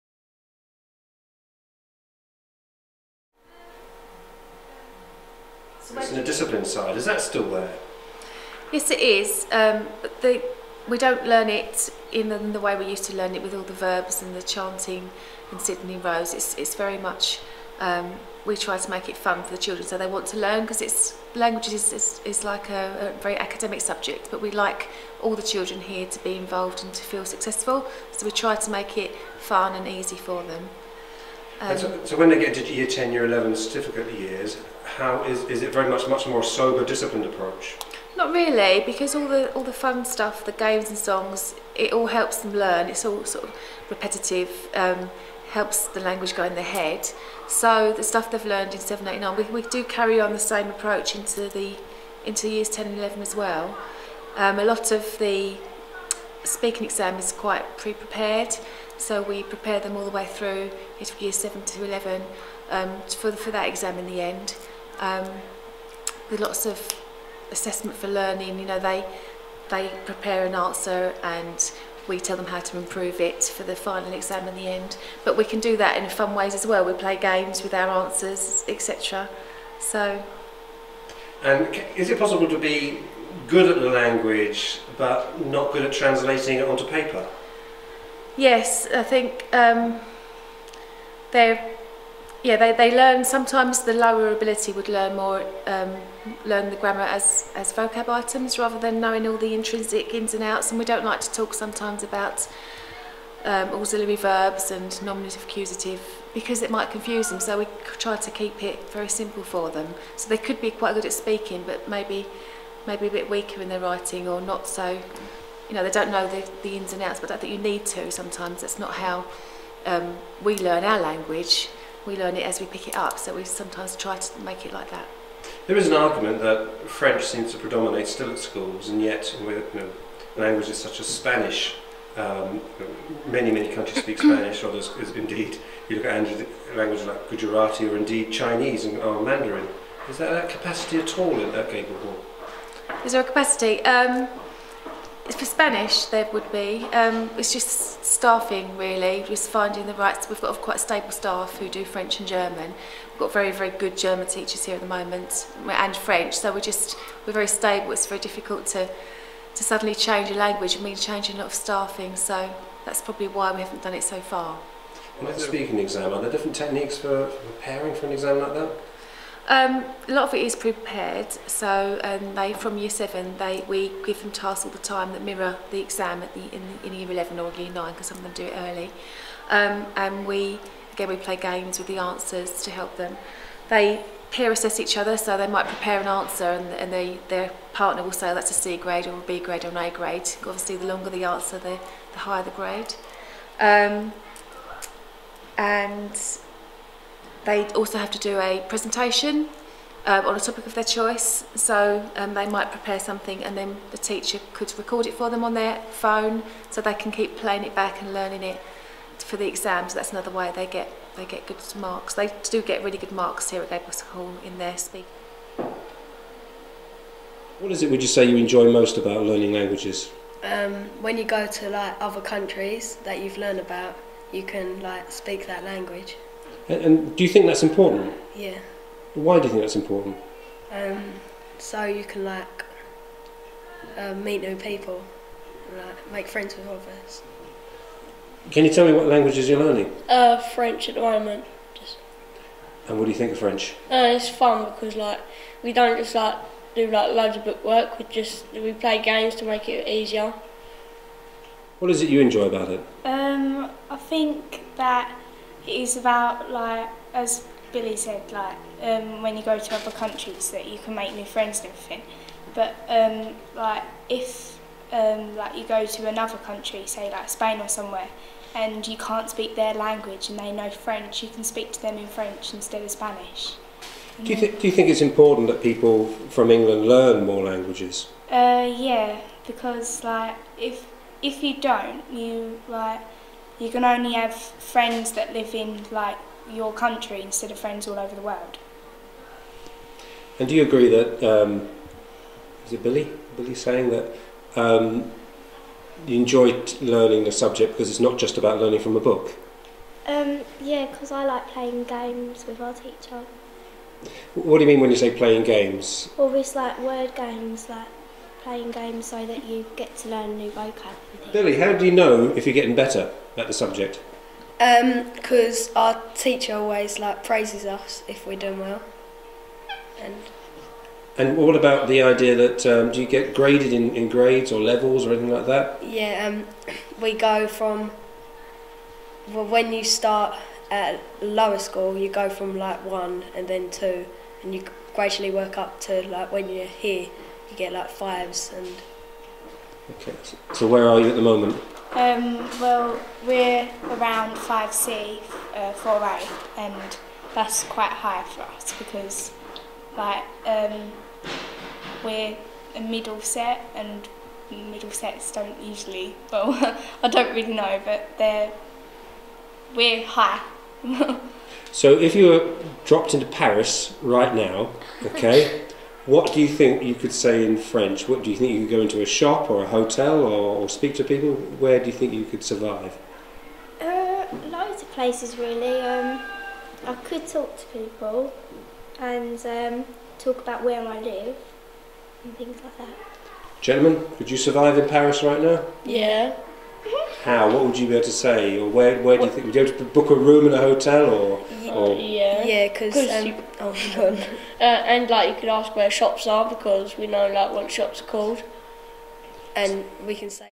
Languages at Gable Hall
How the discipline of teaching language has changed over the years.